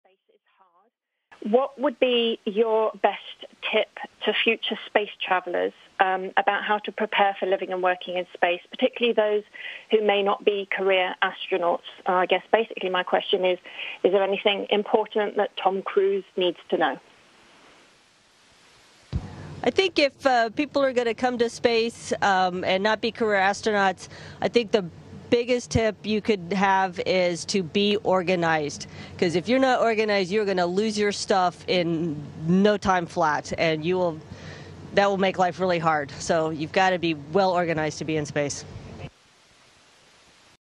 What you need to know if you plan to be a space tourist
NASA astronaut and physicist Shannon Walker, who arrived at the International Space Station while aboard the SpaceX Crew Dragon capsule has a tip to future space tourists.

"I think if people are going to come to space and not be career astronauts then I think the biggest tip you could have is to be organized, because if you are not organized, you are going to lose your stuff in no time flat, and that will make life hard so you got to be well-organized to be in space," Walker said.